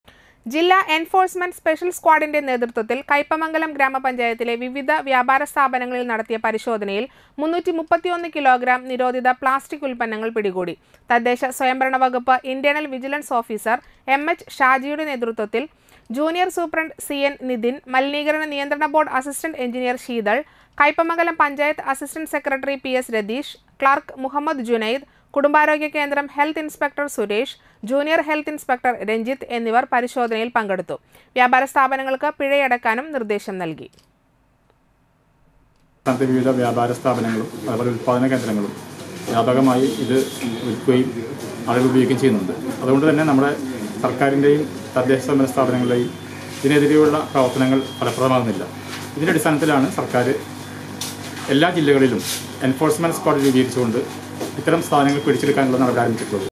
ജില്ലാ എൻഫോഴ്സ്മെൻ്റ് സ്പെഷ്യൽ സ്ക്വാഡിൻ്റെ നേതൃത്വത്തിൽ നിരോധിതപ്ലാസ്റ്റിക്ക് ഉത്പ്പന്നങ്ങൾപിടികൂടി
കയ്പമംഗലം ഗ്രാമ പഞ്ചായത്തിലെ വിവിധ വ്യാപാര സ്ഥാപനങ്ങളിൽ നടത്തിയ പരിശോധനയിൽ 331 കിലോഗ്രാം നിരോധിത പ്ലാസ്റ്റിക്ക് ഉത്പ്പന്നങ്ങൾ പിടികൂടി